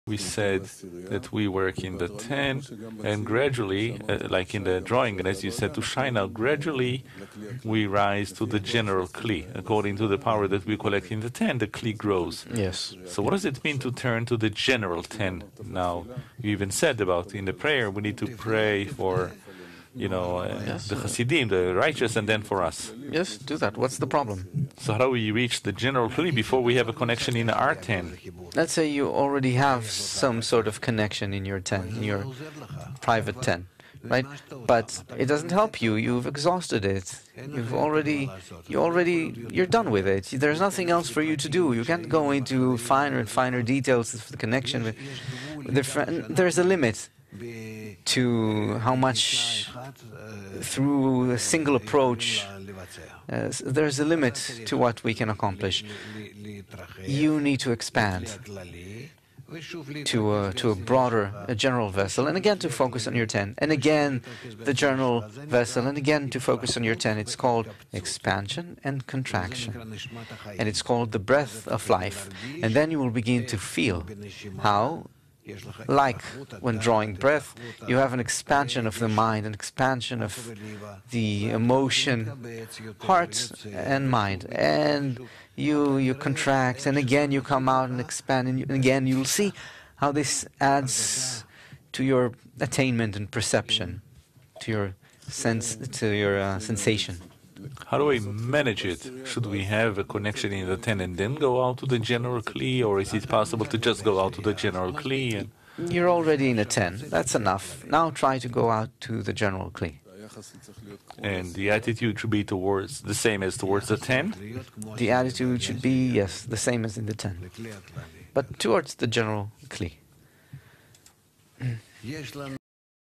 Expansion and Contraction - The Breath of Life - From The Ten go out to the General Kli.
Lesson on the topic of "Correction of the Shattering" (Preparation to the Europe Convention)